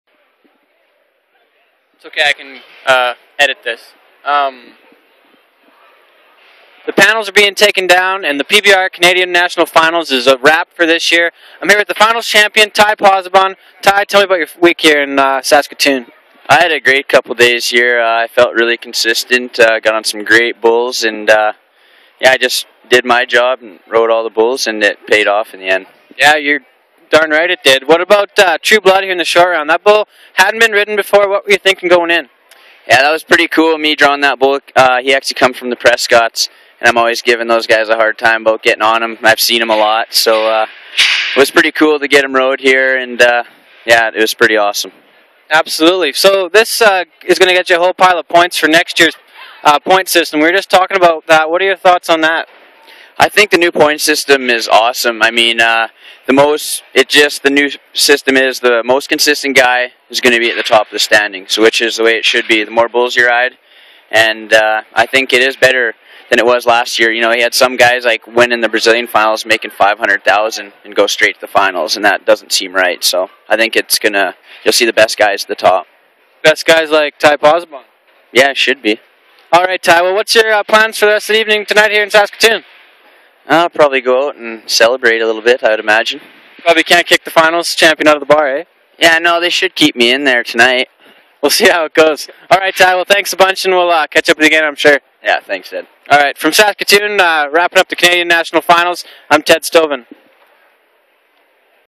Ty Pozzobon PBRCF '12 Champion interview with Ted Stovin
At the PBR Canadian Finals in Saskatoon, SK at the Credit Union Center on November 16th & 17th 2012.

Everything Cowboy is you source for Canadian Rodeo and Bull Riding News.